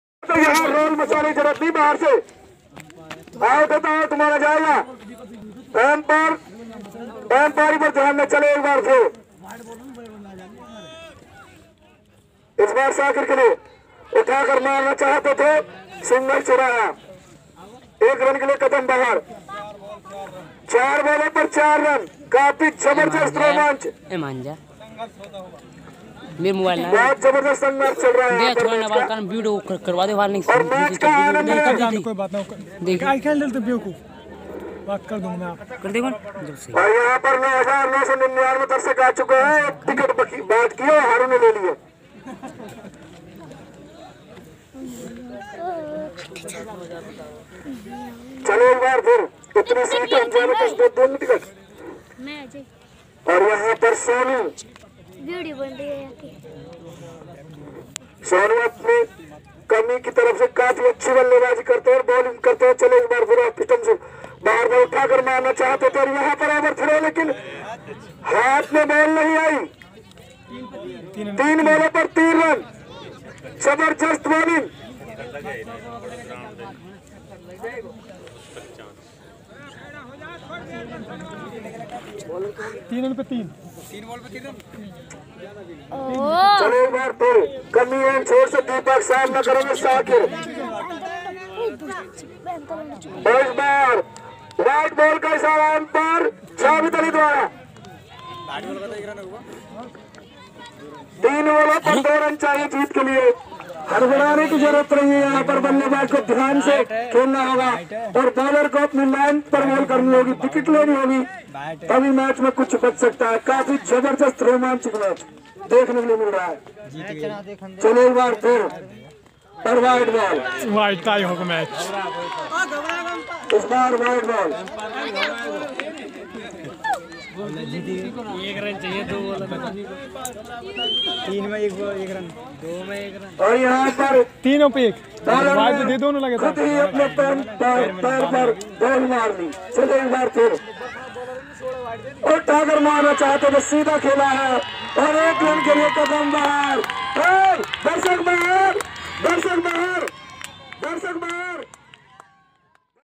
हाथियाका टूर्नामेंट लाइव देखें आज की दूसरी पारी
दोस्तों आपकी जानकारी के लिए बता दें कि UP गांव बरसाना के पास हाथियाका में टूर्नामेंट हो रहा है

मेरा यूट्यूब चैनल

वीडियो एडिटर मोहम्मद शमीम मेवात एंड हरियाणा